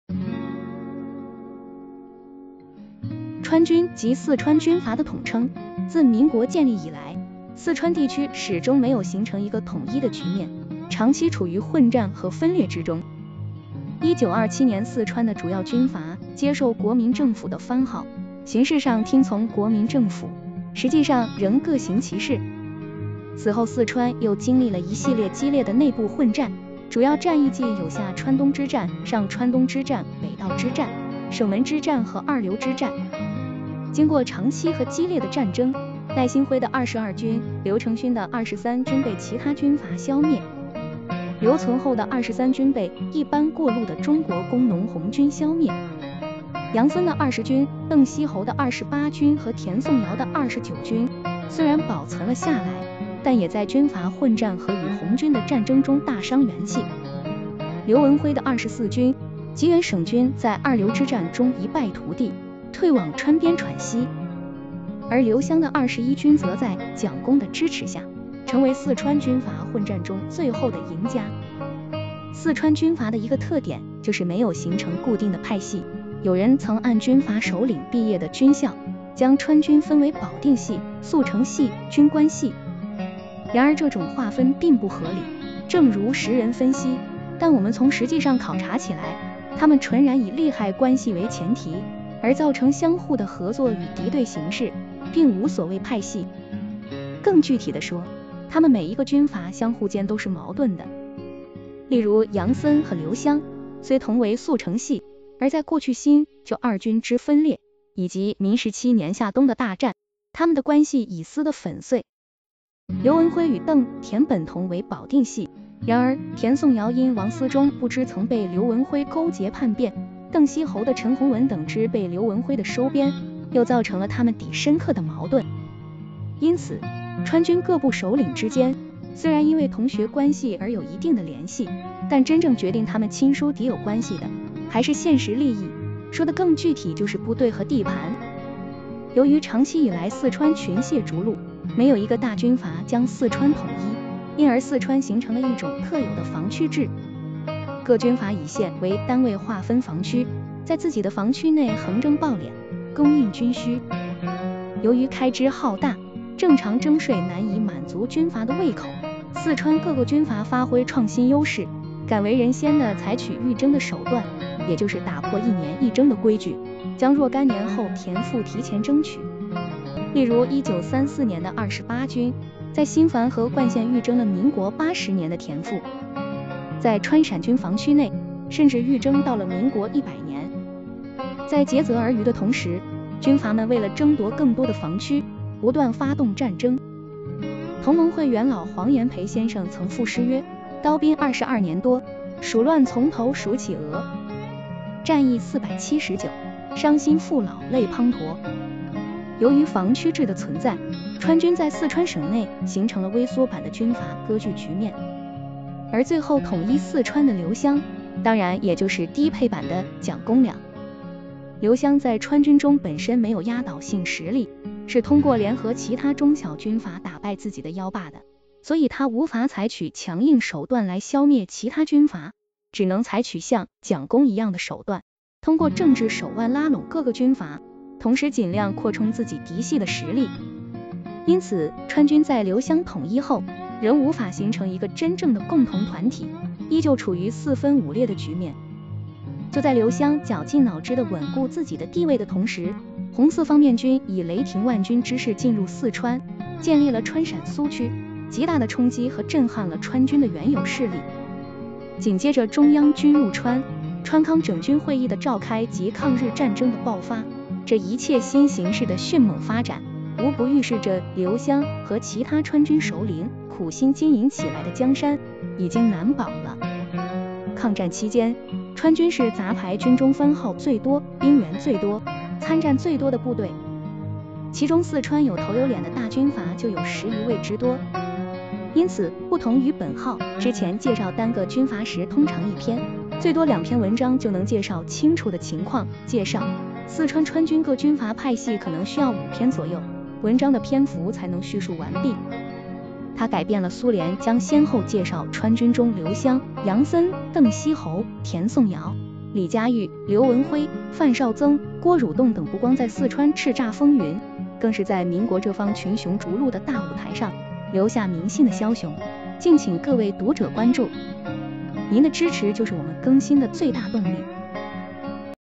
抗战杂牌一箩筐：四川川军（前言与概述）
在二刘之战中一败涂地 退往川边喘息 而刘湘的21军则在 蒋公的支持下 成为四川军阀混战中最后的赢家 四川军阀的一个特点就是没有形成固定的派系 有人曾按军阀首领毕业的军校 将川军分为 四川川军（前言与概述） 川军即四川军阀的统称 自民国建立以来 四川地区始终没有形成一个统一的局面 长期处于混战和分裂之中 1927年四川的主要军阀接受国民政府的番号 形式上听从国民政府 实际上仍各行其是 此后四川又经历了一系列激烈的内部混战 主要战役计有下川东之战、上川东之战、北道之战、省门之战和二刘之战 经过长期和激烈的战争 赖心辉的22军、刘成勋的23军被其他军阀消灭 刘存厚的23军被一般过路的中国工农红军消灭 杨森的20军、邓锡侯的28军和田颂尧的29军虽然保存了下来 但也在军阀混战和与红军的战争中大伤元气 刘文辉的24军 即原 四川的刘湘 当然也就是低配版的 蒋公了 刘湘在川军中本身没有压倒性实力 是通过联合其他中小军阀打败自己的 但我们从实际上考察起来 他们纯然以利害关系为前提 而造成相互的合作与敌对形式 并无所谓派系 更具体地说 他们每一个军阀相互间都是矛盾的 例如杨森和刘湘 虽同为速成系 而在过去新、旧二军之分裂 以及民十七年下东的大战 他们的关系已撕得粉碎 刘文辉与邓、田本同为保定系 然而田颂尧因王思忠部之曾被刘文辉勾结叛变 邓锡侯的陈鸿文等之被刘文辉的收编 又造成了他们底深刻的矛盾 的手段 也就是打破一年一征的规矩 将若干年后田赋提前征取 例如1934年的28军 在新繁和灌县预征了民国80年的田赋 在川陕军防区内 甚至预征到了民国100年 在竭泽而渔的同时 军阀们为了争夺更多的防区 不断发动战争 同盟会元老黄炎培先生曾赋诗曰 后仍无法形成一个真正的共同团体 依旧处于四分五裂的局面 就在刘湘绞尽脑汁地稳固自己的地位的同时 红四方面军以雷霆万钧之势进入四川 建立了川陕苏区 极大地冲击和震撼了川军的原有势力 紧接着中央军入川、川康整军会议的召开及抗日战争的爆发 这一切新形势的迅猛发展 无不预示着刘湘和其他川军首领苦心经营起来的江山已经难保了 抗战期间 川军是杂牌军中番号最多、兵员最多、参战最多的部队 其中四川有头有脸的